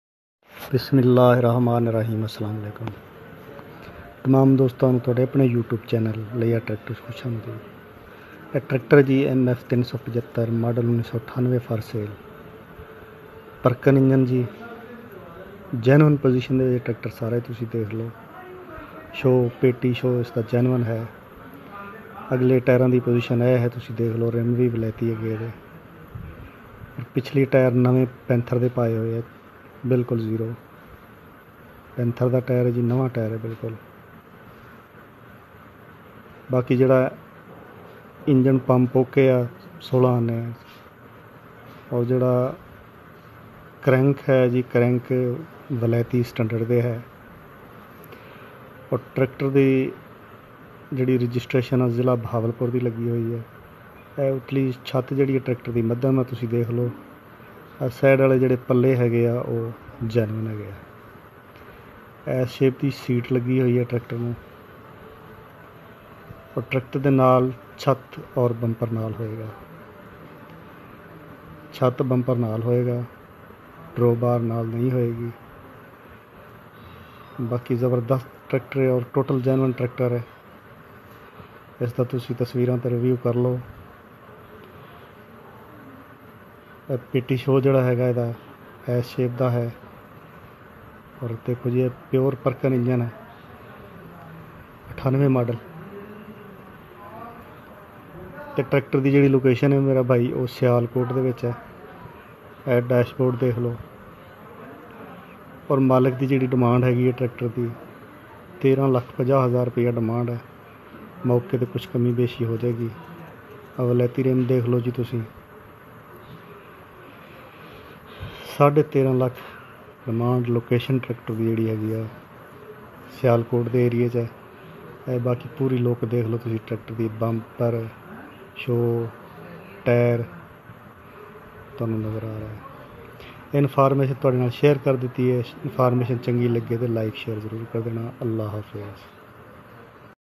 Used Impoted Tractor MF.375 perkin model 1998 for sale|| Layyah Tractors
This video Used Tractor MF.375 perkin  1998 For sale in sialkot pakistan .75 Hp second Hand Traktor  MF.375  perkin Model 1998 For sale in uper punjab Pakistan . According to owner Overall condition of Tractor good.Grnl Tyre installed .Self start.genuine condition.Genuine engine total Genuine.Location sialkot.Price 1350000.call / W.app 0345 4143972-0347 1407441